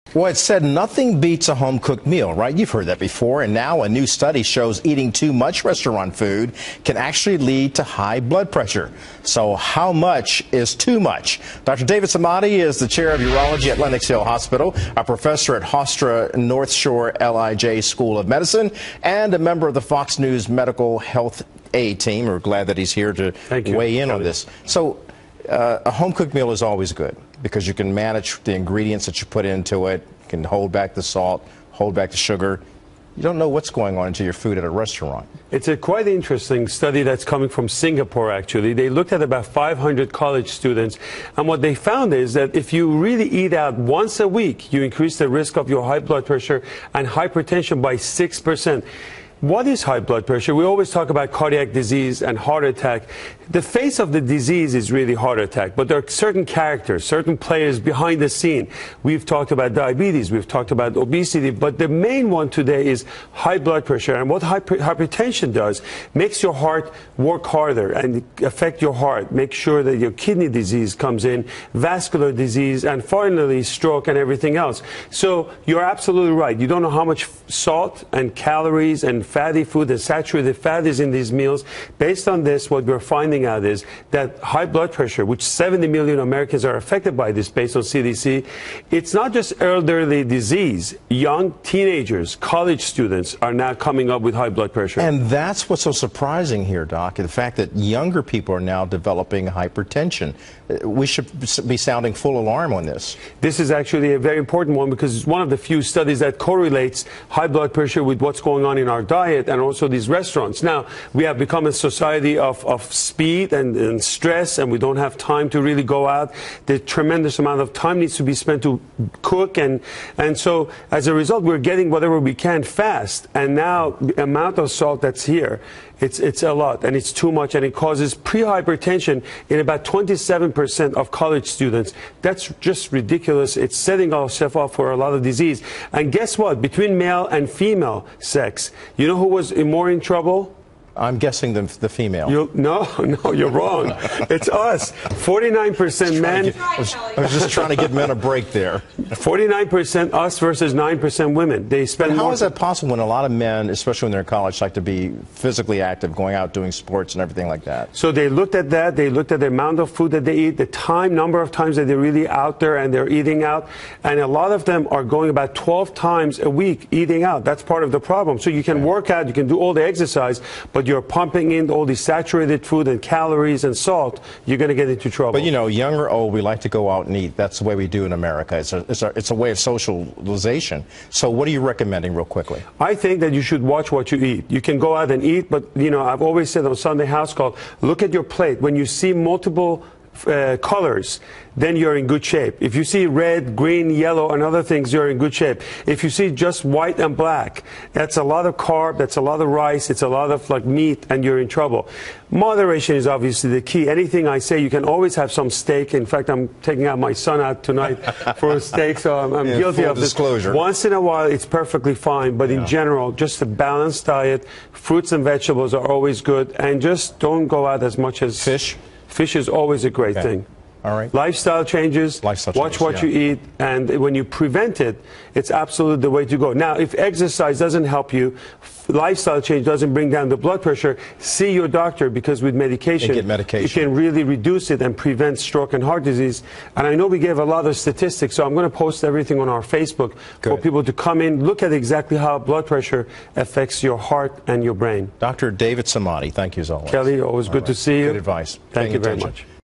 The dangers of eating too much restaurant food
Study links dining out to increased risk of hypertension, heart disease
Watch Dr David Samadi and Kelly Wright talk about Healthy Living and Heart Health.